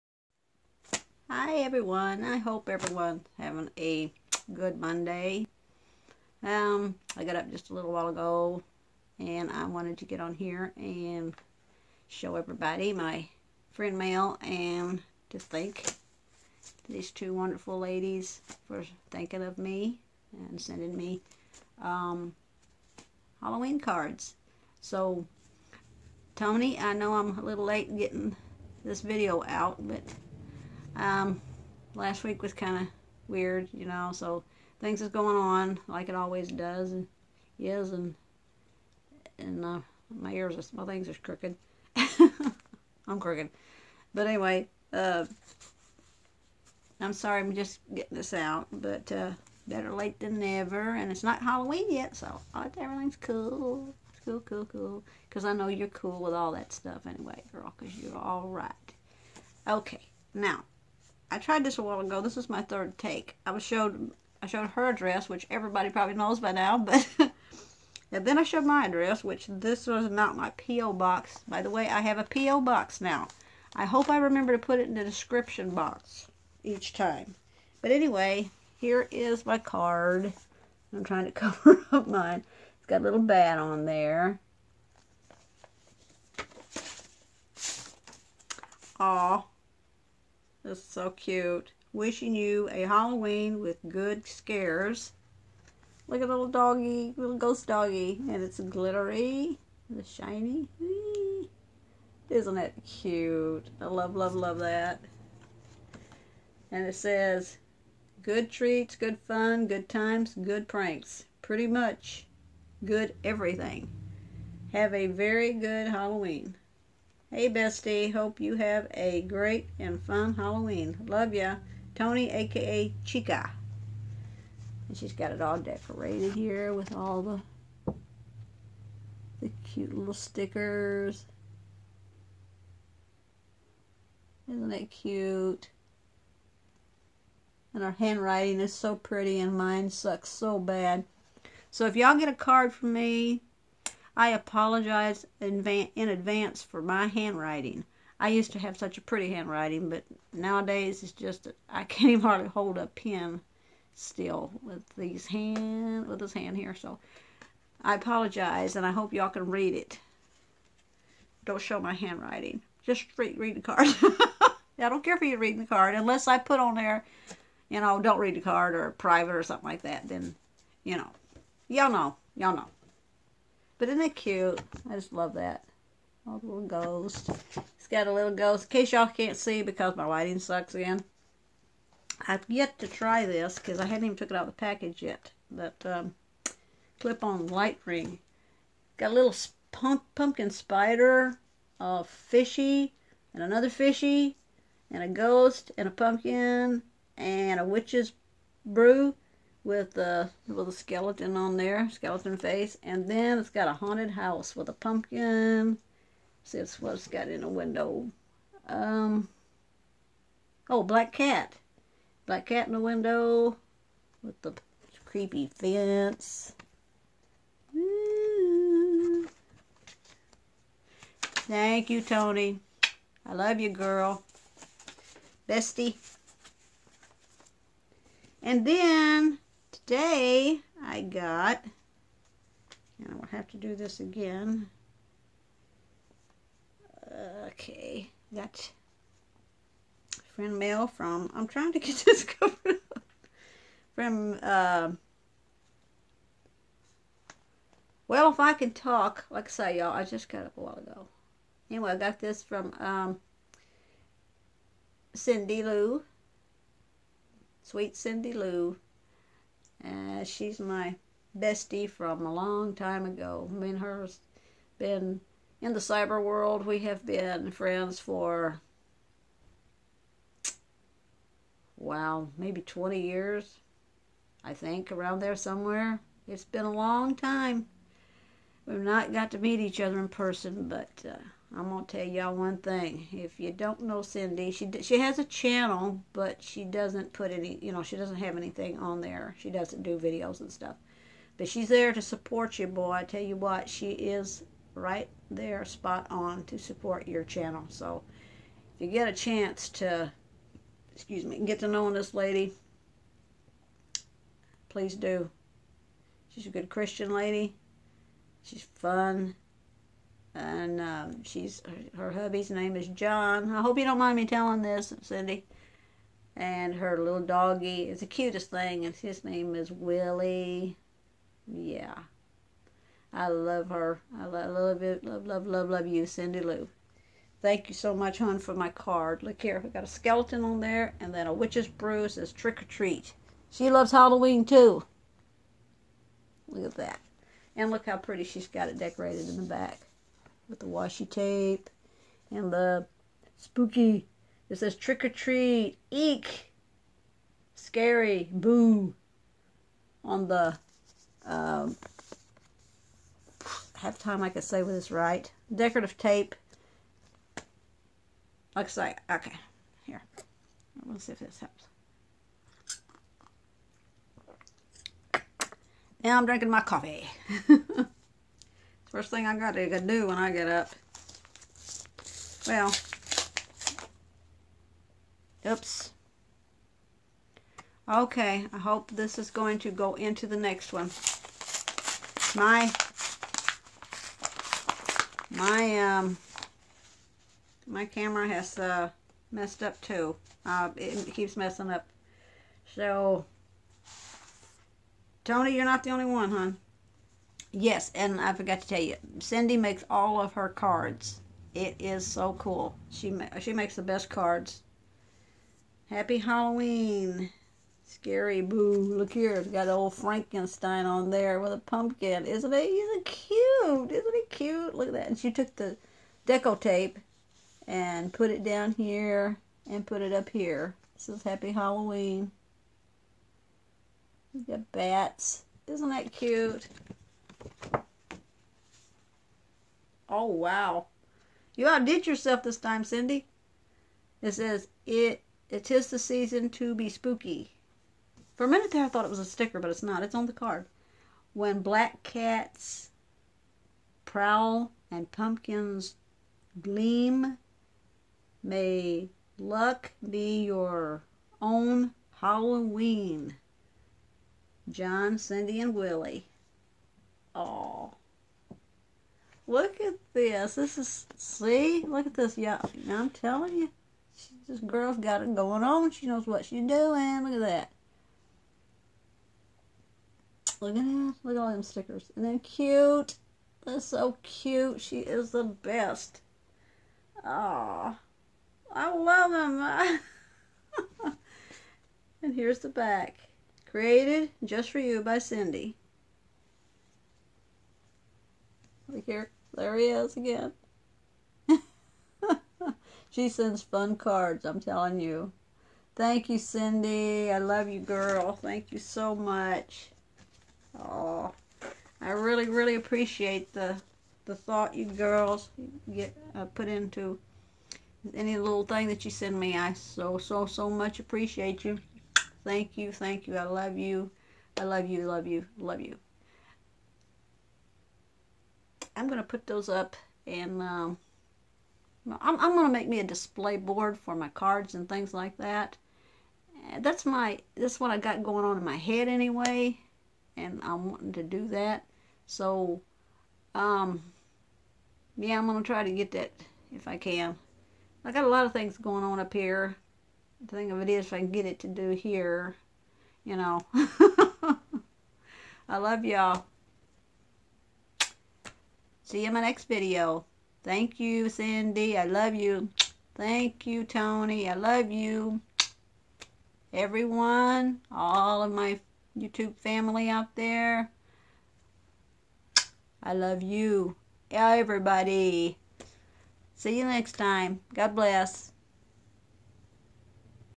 Fun Friend Mail! 🎃 💞💞
Awesome friend mail from 2 great ladies! 🌹🌹
I will link Creative Chica Hauls if I can figure out how to do that on my phone. I haven't tryed to do that yet! I'm still learning y'all!!😉👌🏼
I'm not sure if Cindy wants me to link her channel, so I will ask before I do.

Cindy Lou

I love you BFF, aka Toni! ❤
I love you Sis, aka Cindy! ❤
Thank you sweet gals for the fun Halloween cards!!